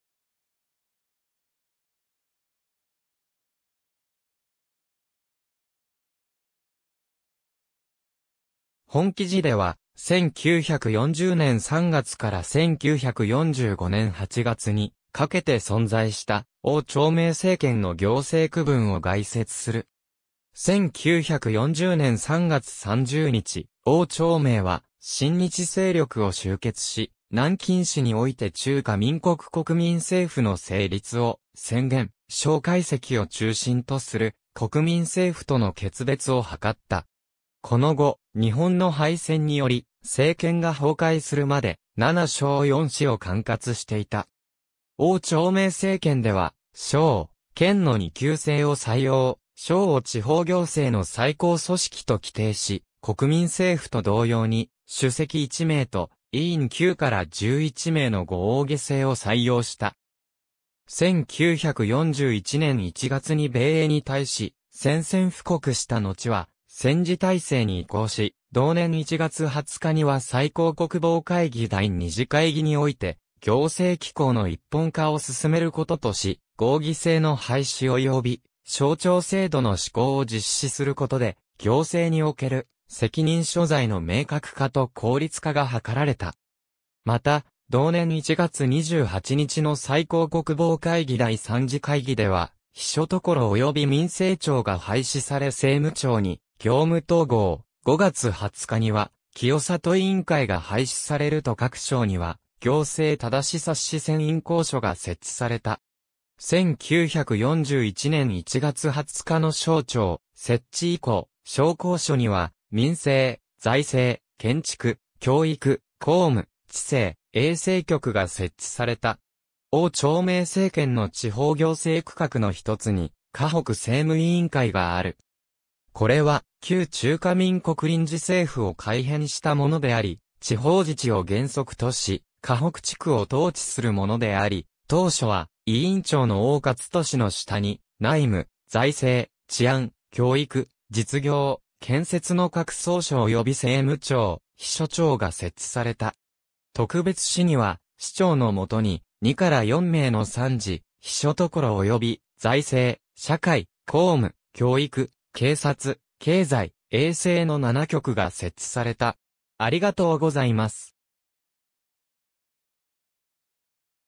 汪兆銘政権の行政区分
本記事では、1940年（民国29年）3月から1945年（民国34年）8月にかけて存在した汪兆銘政権（汪兆銘南京政府）の行政区分を概説する。
1940年3月30日、汪兆銘は親日勢力を集結し南京市において中華民国国民政府の成立を宣言、蔣介石を中心とする国民政府との決別を図った。
この後、日本の敗戦により政権が崩壊するまで、7省4市を管轄していた。
汪兆銘政権では省・県の二級制を採用、省を地方行政の最高組織と規定し国民政府と同様に、主席1名と委員9から11名の合議制を採用した。
1941年（民国30年）1月に米英に対し宣戦布告したのちは戦時体制に移行し、同年1月20日には最高国防会議第2次会議において行政機構の一本化を進めることとし、合議制の廃止及び省長制度の施行を実施することで、行政における責任所在の明確化と効率化が図られた。
また同年1月28日の最高国防会議第3次会議では秘書処及び民政庁が廃止され政務庁に業務統合、5月20日には清郷委員会が廃止されると各省には行政督察専員公署が設置された。
1941年（民国30年）1月20日の省長設置以降、省公署には民政、財政、建築、教育、工務、地政、衛生局が設置された。
汪兆銘政権の地方行政区画のひとつに華北政務委員会がある。
これは旧中華民国臨時政府を改編したものであり、地方自治を原則とし華北地区を統治するものであり、当初は委員長の王克敏の下に、内務、財政、治安、教育、実業、建設の各総署及び政務庁、秘書庁が設置された。
特別市には市長の下に、2から4名の参事、秘書処及び財政、社会、工務、教育、警察、経済、衛生の7局が設置された。